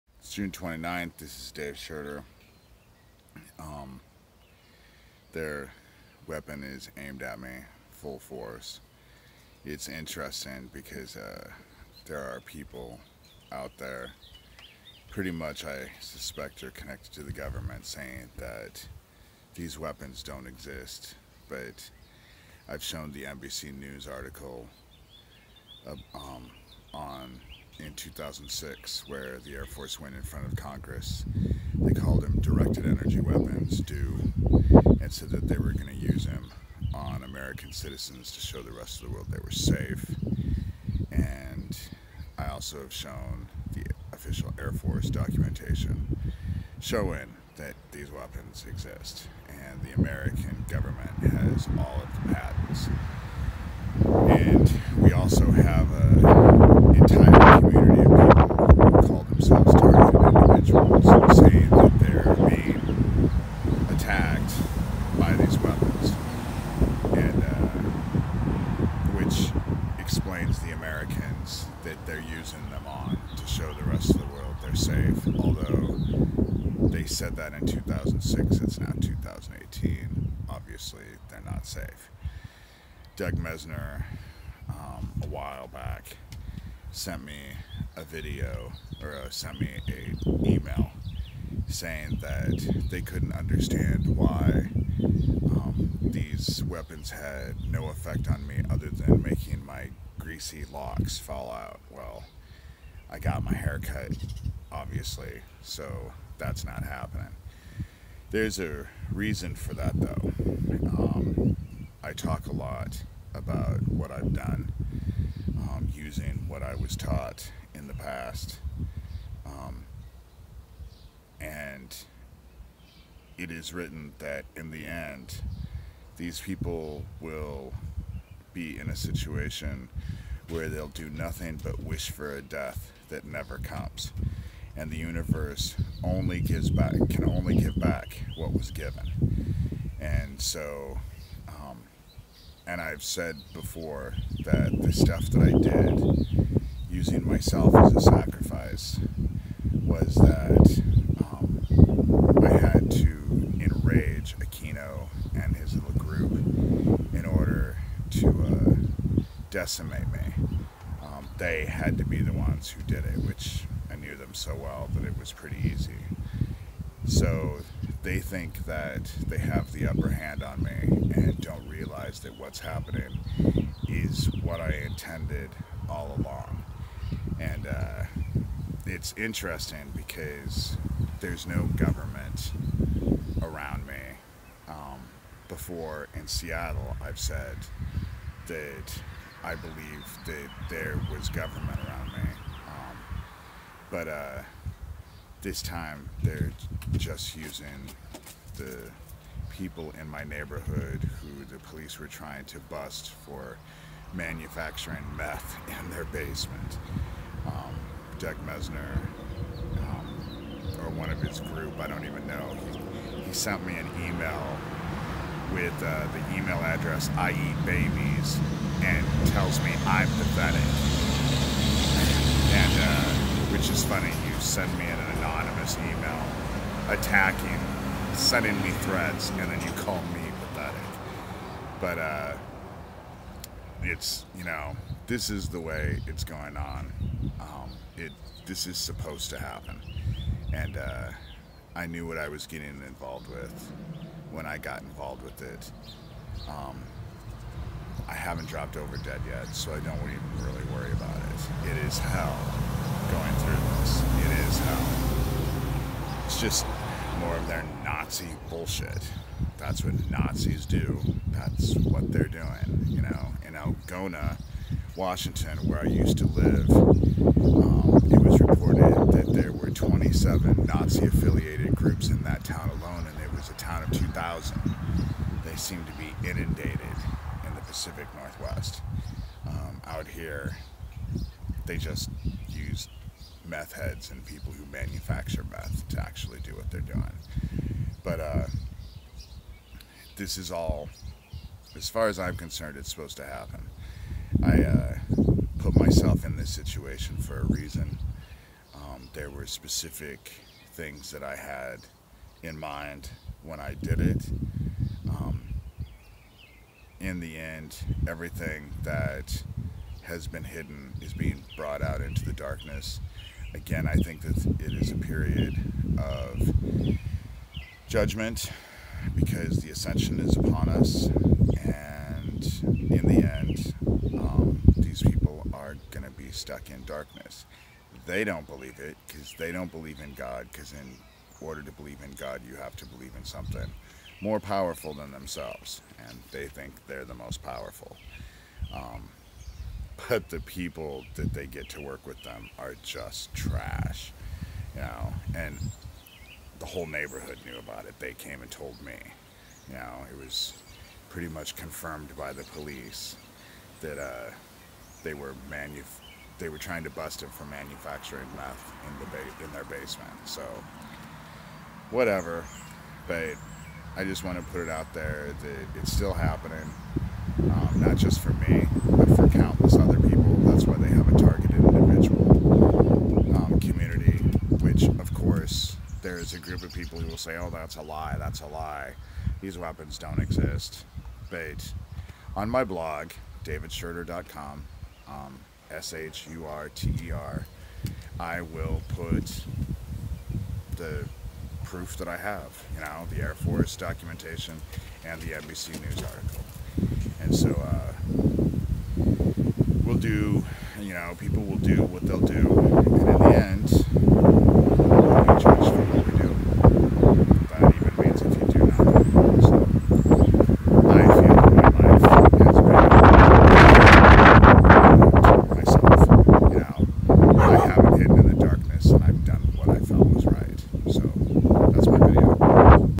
Our Govt's DEW Weapon Attacking Me Sucks- But In the End I Lived a Life Worth Living
Again- due to the trolls on top of me- I won't let comments.
DavidShurter.com